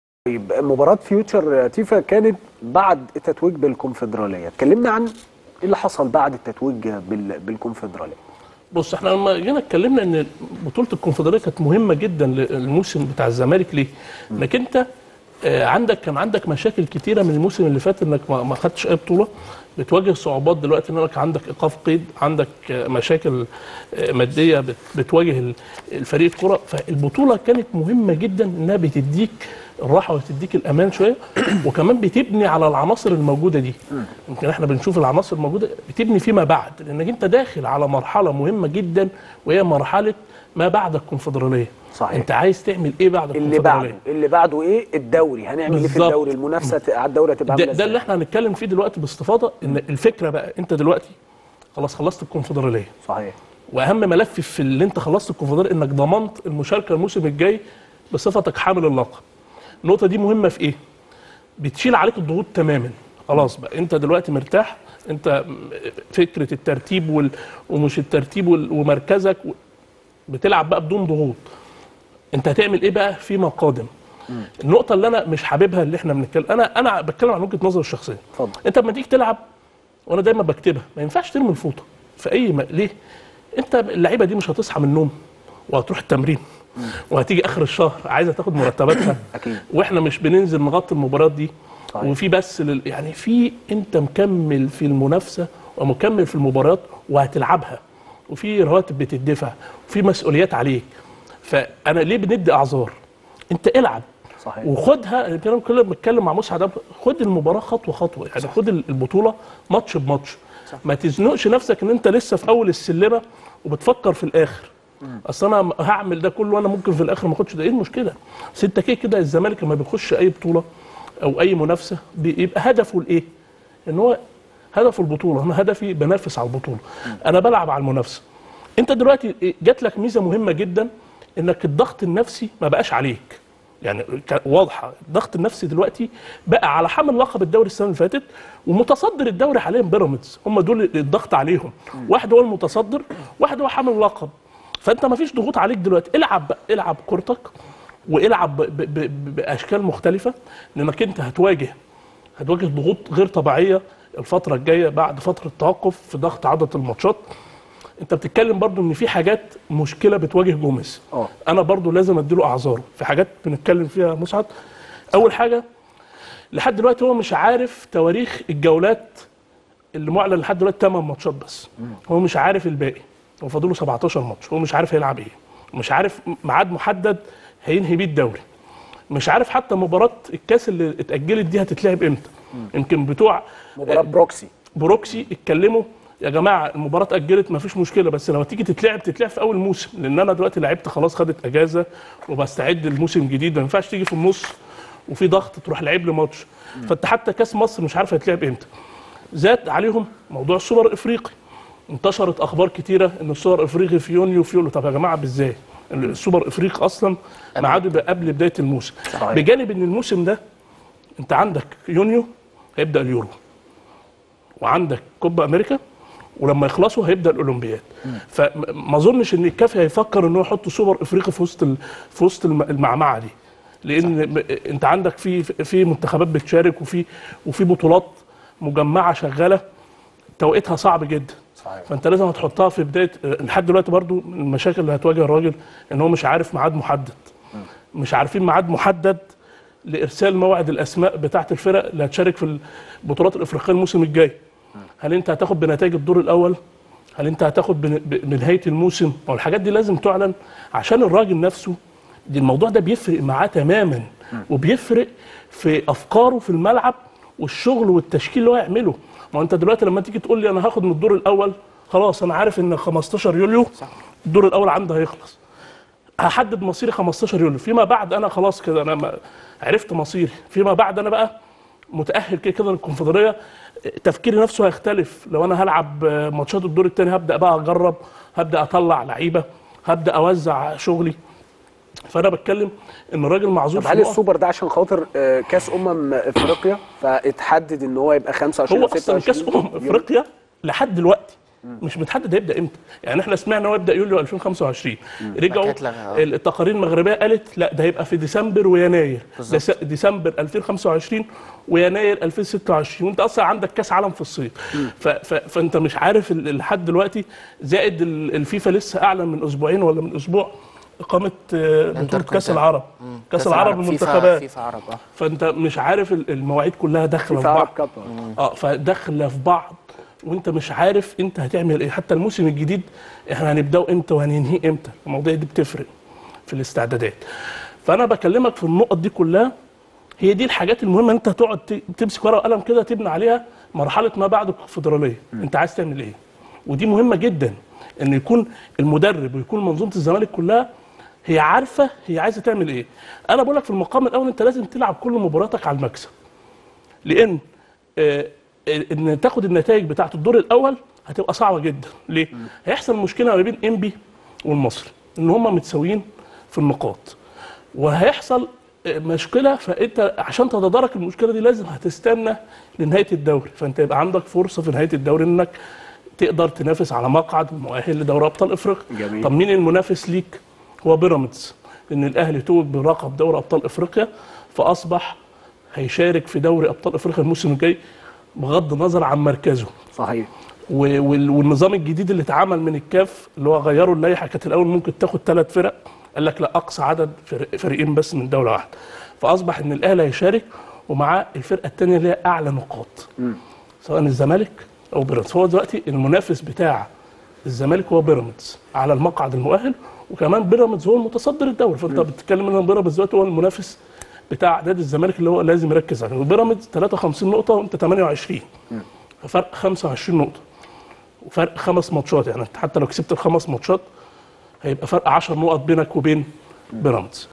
عبداللطيف فوزي: لا يوجد ضغط على لاعبي فريق الزمالك في الدوري ويجب على اللاعبين التركيز في كل مباراة
عبداللطيف فوزي: لا يوجد ضغط على لاعبي فريق الزمالك في الدوري ويجب على اللاعبين التركيز في كل مباراة زملكاوي"